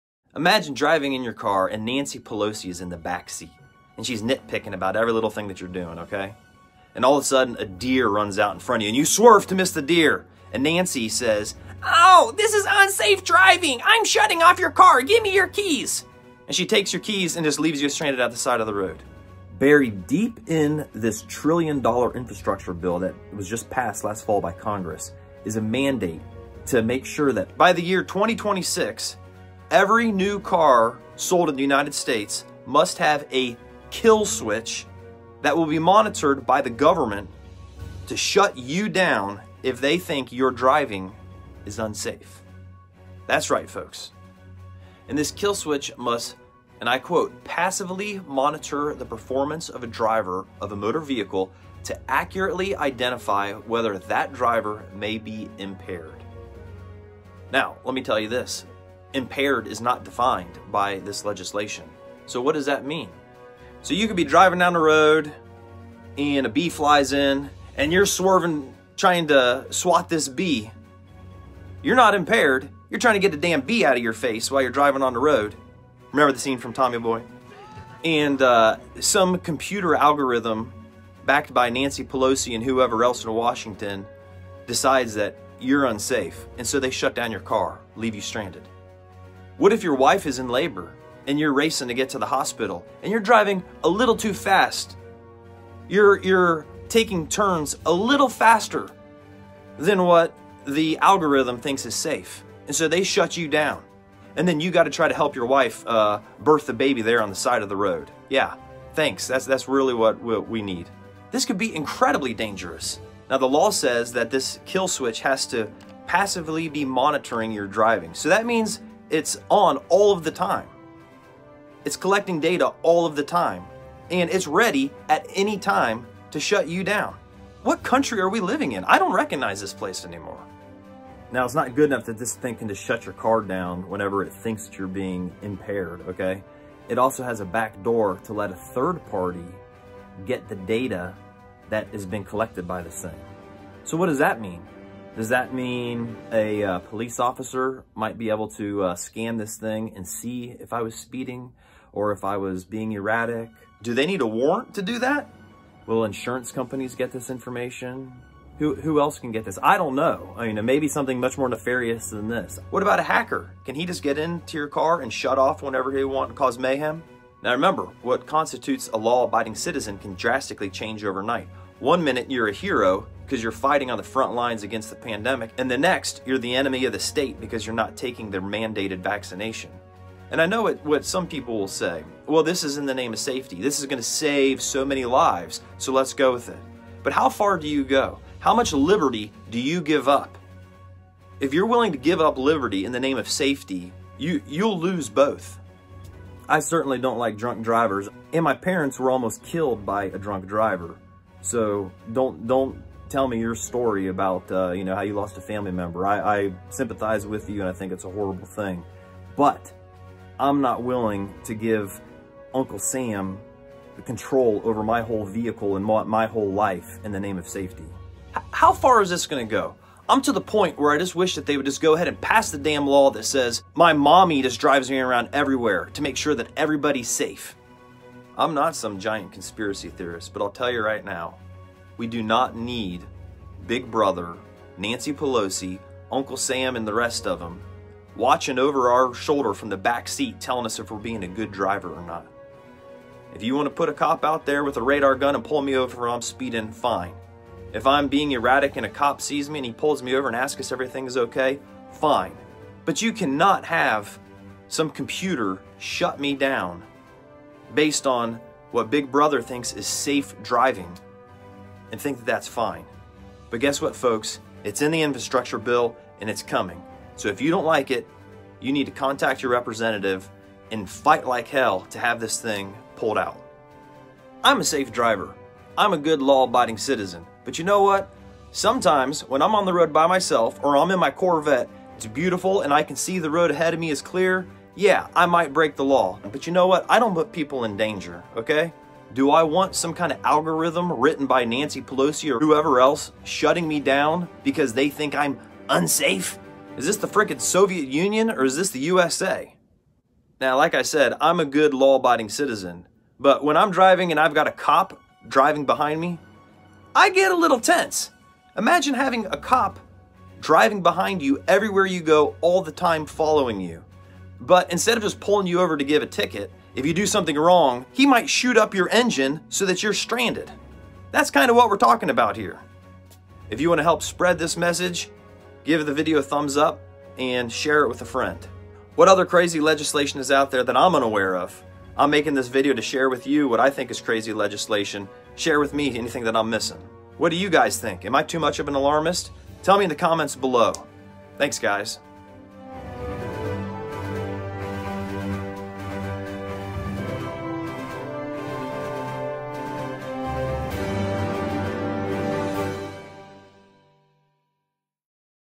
MANDATORY Car Kill Switch Law Passed by Congress! Stop Nancy Pelosi NOW!
Share this with every red-blooded American before it's too late!